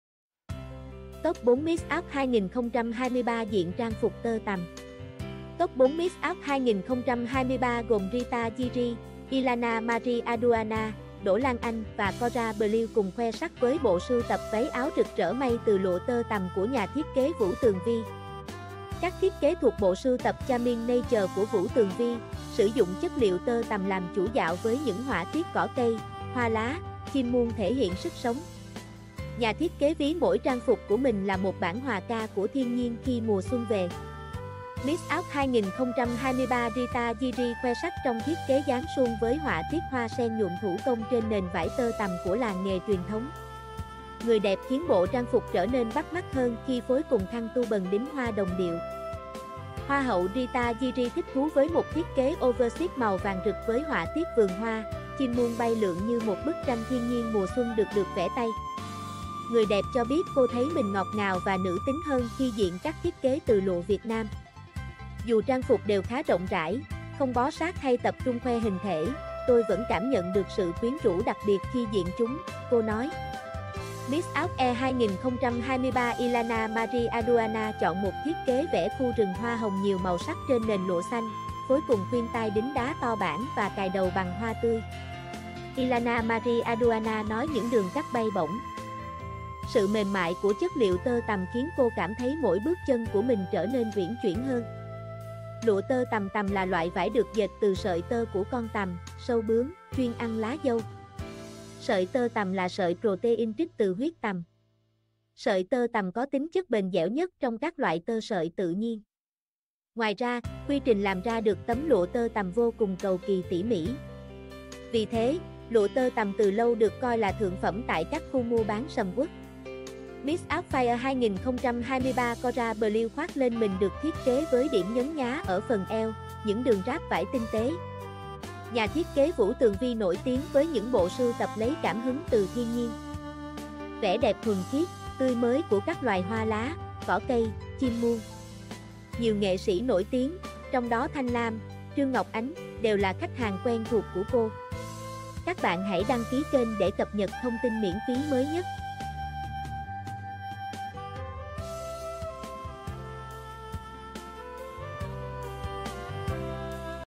Top 4 Miss Earth 2023 diện trang phục tơ tằm
Top 4 Miss Earth 2023 gồm Drita Ziri, Yllana Marie Aduana, Đỗ Lan Anh và Cora Bliault cùng khoe sắc với bộ sưu tập váy áo rực rỡ may từ lụa tơ tằm của NTK Vũ Tường Vi.

Các thiết kế thuộc bộ sưu tập Charming Nature của Vũ Tường Vi, sử dụng chất liệu tơ tằm làm chủ dạo với những họa tiết cỏ cây, hoa lá, chim muông thể hiện sức sống. Nhà thiết kế ví mỗi trang phục của mình là một bản hòa ca của thiên nhiên khi mùa xuân về.

Miss Earth 2023 Drita Ziri khoe sắc trong thiết kế dáng suông với họa tiết hoa sen nhuộm thủ công trên nền vải tơ tằm của làng nghề truyền thống. Người đẹp khiến bộ trang phục trở nên bắt mắt hơn khi phối cùng khăn turban đính hoa đồng điệu.

Hoa hậu Drita Ziri thích thú với một thiết kế oversized màu vàng rực với họa tiết vườn hoa, chim muông bay lượn như một bức tranh thiên nhiên mùa xuân được được vẽ tay. Người đẹp cho biết cô thấy mình ngọt ngào và nữ tính hơn khi diện các thiết kế từ lụa Việt Nam. Dù trang phục đều khá rộng rãi, không bó sát hay tập trung khoe hình thể, tôi vẫn cảm nhận được sự quyến rũ đặc biệt khi diện chúng, cô nói.

Miss Earth Air 2023 Yllana Marie Aduana chọn một thiết kế vẽ khu rừng hoa hồng nhiều màu sắc trên nền lụa xanh, phối cùng khuyên tai đính đá to bản và cài đầu bằng hoa tươi.

Yllana Marie Aduana nói những đường cắt bay bổng, sự mềm mại của chất liệu tơ tằm khiến cô cảm thấy mỗi bước chân của mình trở nên uyển chuyển hơn.

Lụa tơ tằmtơ tằm là loại vải được dệt từ sợi tơ của con tằm (sâu bướm) chuyên ăn lá dâu. Sợi tơ tằm là sợi protein trích từ huyết tằm. Sợi tơ tằm có tính chất bền dẻo nhất trong các loại tơ sợi tự nhiên. Ngoài ra,quy trình làm ra được tấm lụa tơ tằmvô cùng cầu kỳ tỉ mỉ. Vì thế, lụa tơ tằm từ lâu được coi là thượng phẩm tại các khu mua bán sầm uất.

Miss Earth Fire 2023 Cora Bliault khoác lên mình được thiết kế với điểm nhấn nhá ở phần eo, những đường ráp vải tinh tế.

Nhà thiết kế Vũ Tường Vi nổi tiếng với những bộ sưu tập lấy cảm hứng từ thiên nhiên, vẻ đẹp thuần khiết, tươi mới của các loài hoa lá, cỏ cây, chim muông. Nhiều nghệ sĩ nổi tiếng, trong đó Thanh Lam, Trương Ngọc Ánh... đều là khách hàng quen thuộc của cô.